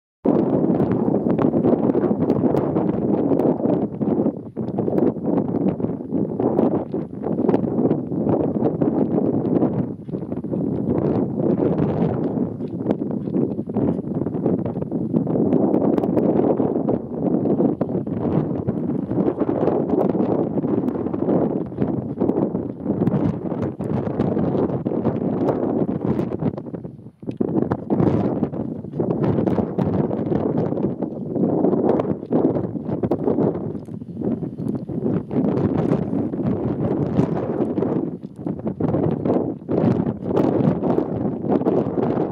Gepard zerfleischt Antilope
Gepard frisst Antilope bis die Organe zum Vorschein kommen. Selbst erlebt in der Maasai Mara. Das willst du auch?